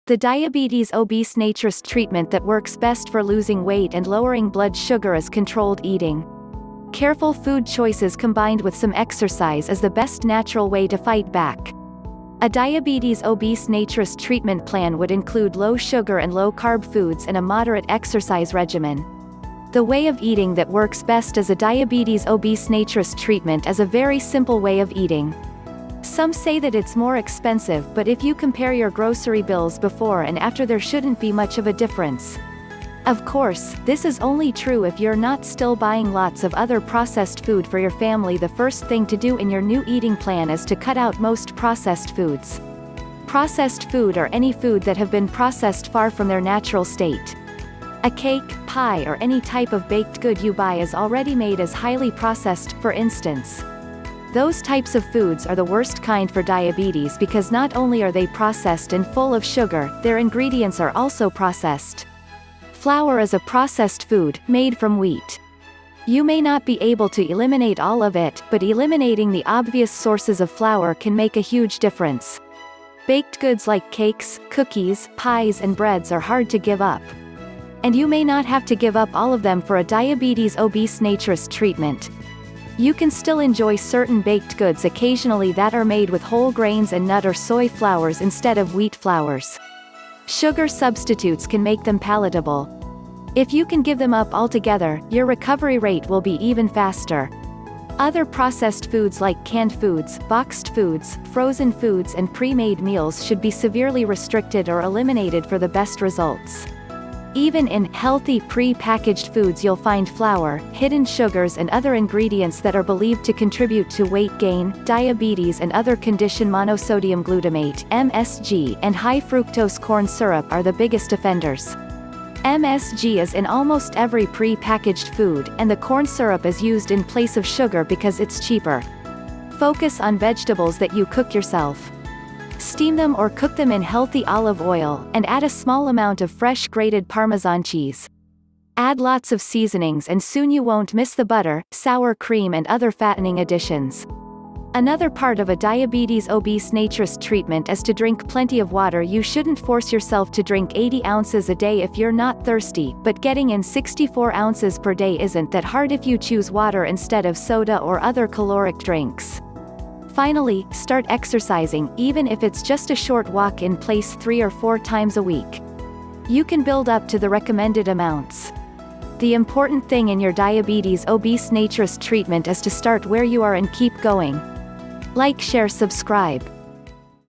3 step diabetes obese naturist treatment | natural diabetes treatment
In this video you will find the best 3 step diabetes obese naturist treatment. The best natural diabetes treatment for obese kids or adults.

The diabetes obese naturist treatment that works best for losing weight and lowering blood sugar is controlled eating. Careful food choices combined with some exercise is the best natural way to fight back. A diabetes obese naturist treatment plan would include low-sugar and low-carb foods and a moderate exercise regimen.
The way of eating that works best as a diabetes obese naturist treatment is a very simple way of eating. Some say that it's more expensive, but if you compare your grocery bills before and after there shouldn't be much of a difference. Of course, this is only true if you're not still buying lots of other processed foods for your family members.

The first thing to do in your new eating plan is to cut out most processed foods. Processed foods are any foods that have been processed far from their natural state. A cake, pie or any type of baked good you buy already made is highly processed, for instance.

Those types of foods are the worst kind for diabetes because not only are they processed and full of sugar, their ingredients are also processed. Flour is a processed food, made from wheat. You may not be able to eliminate all of it, but eliminating the obvious sources of flour can make a huge difference.

Baked goods like cakes, cookies, pies and breads are hard to give up. And you may not have to give up all of them for a diabetes obese naturist treatment. You can still enjoy certain baked goods occasionally that are made with whole grains and nut or soy flours instead of wheat flours. Sugar substitutes can make them palatable.

If you can give them up altogether, your recovery rate will be even faster. Other processed foods like canned foods, boxed foods, frozen foods and pre-made meals should be severely restricted or eliminated for the best results.

Even in "healthy" pre-packaged foods you'll find flour, hidden sugars and other ingredients that are believed to contribute to weight gain, diabetes and other conditions. Monosodium glutamate (MSG) and high-fructose corn syrup are the biggest offenders. MSG is in almost every pre-packaged food, and the corn syrup is used in place of sugar because it's cheaper.

Focus on vegetables that you cook yourself. Steam them or cook them in healthy olive oil, and add a small amount of fresh grated parmesan cheese. Add lots of seasonings and soon you won't miss the butter, sour cream and other fattening additions.

Another part of a diabetes obese naturist treatment is to drink plenty of water. You shouldn't force yourself to drink 80 oz a day if you're not thirsty, but getting in 64 oz per day isn't that hard if you choose water instead of soda or other caloric drinks.

Finally, start exercising, even if it's just a short walk in place three or four times a week. You can build up to the recommended amounts. The important thing in your diabetes obese naturist treatment is to start where you are and keep going.